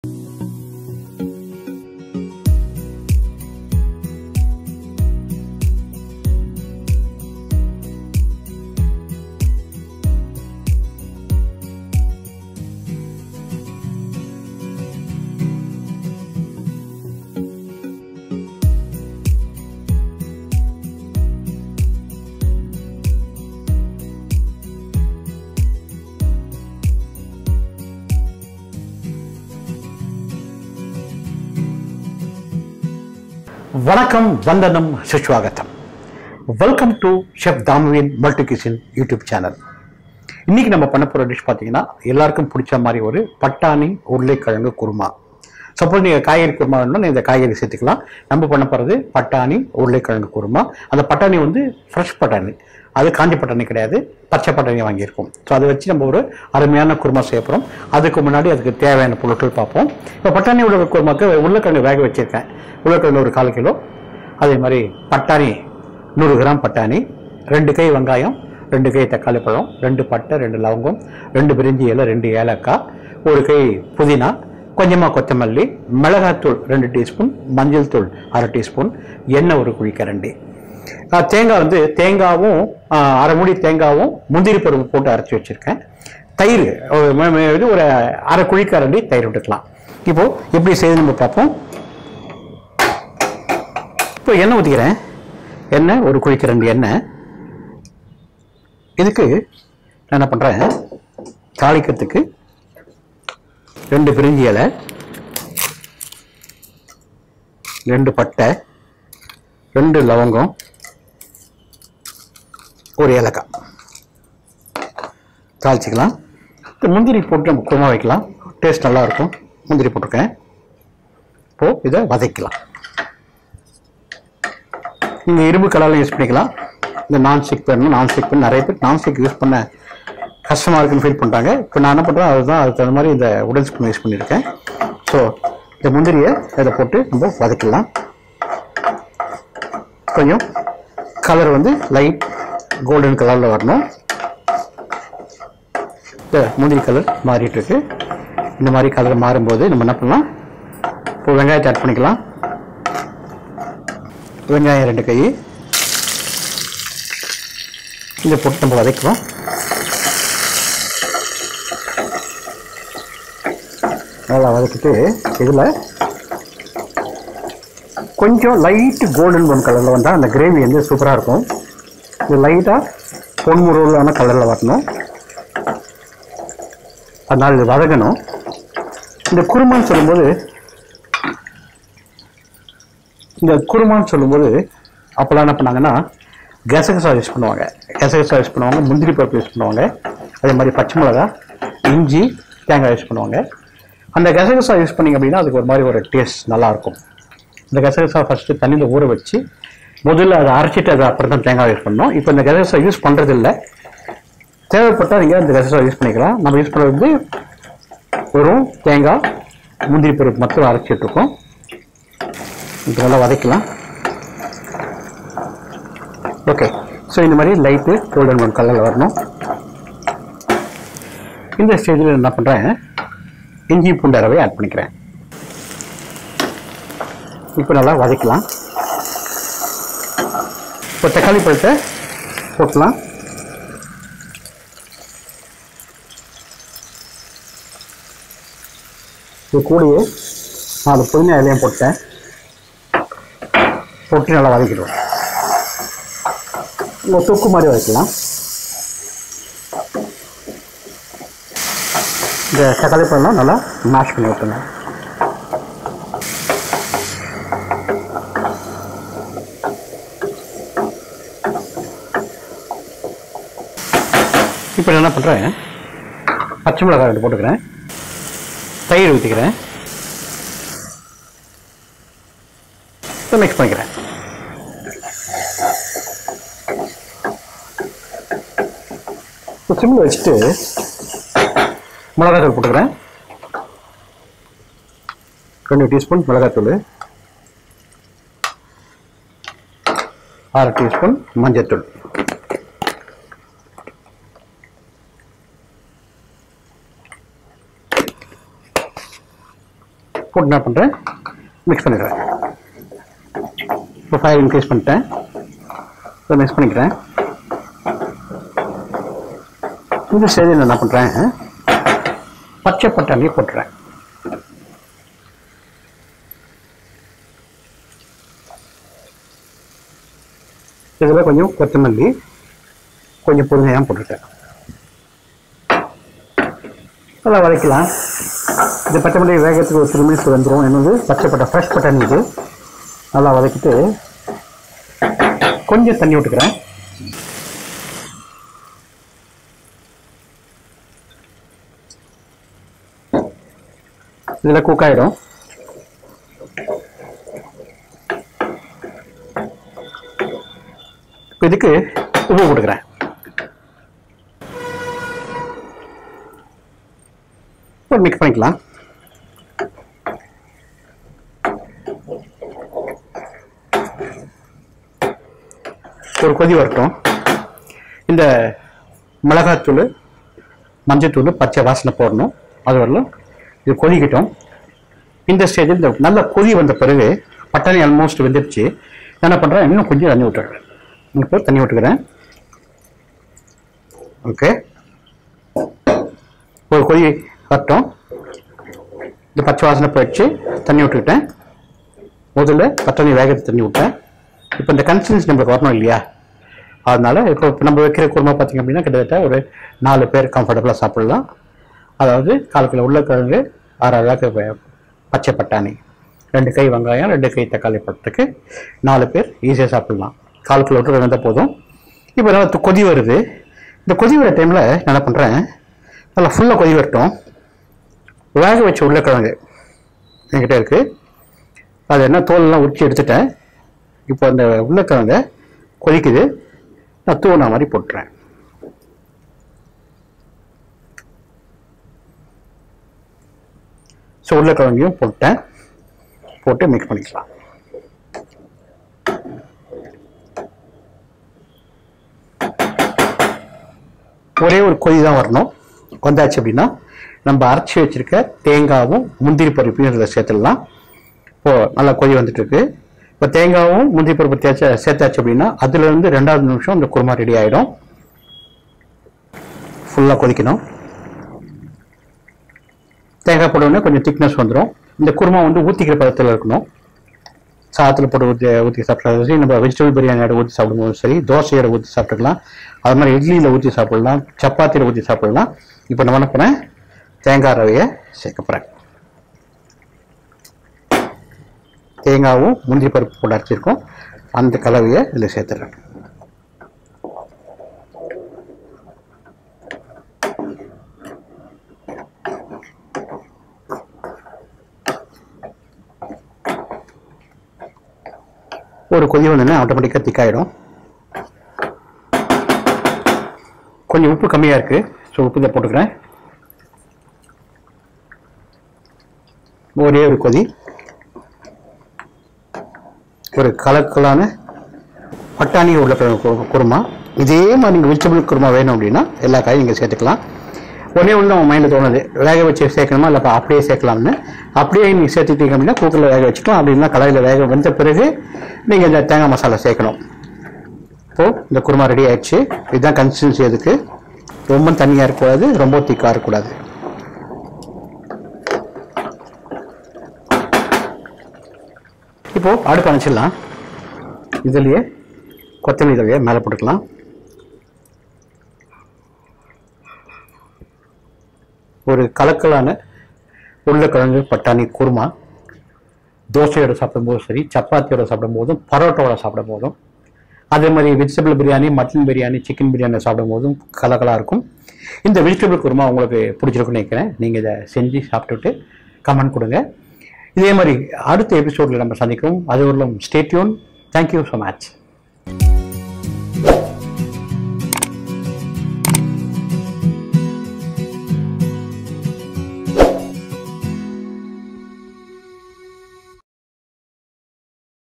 Urulai& Pacha Pattani Khorma ( Fresh Peas & Potato Khorma)
Onion 150 GMs ( 2 handful ) tomatoes 150 (2 handful) green chillies 3 no’s ,potatoes 250 GMs, green peas fresh 100 GMs, curd 1/2 cup , mint leaves 10 leaves (1 hand full ) , coriander leaves little , chili powder 2 teaspoon, turmeric powder 1/2 teaspoon,oil 1 round spoon, salt to taste ,coconut 1/2 no , cashew nuts 10 no’s, cinnamon 2 no’s, bayleaf 2 no’s ,cloves 2 no’s, cardamom 2 no’s(option)

All the contents & Copyrights reserved to chef Damu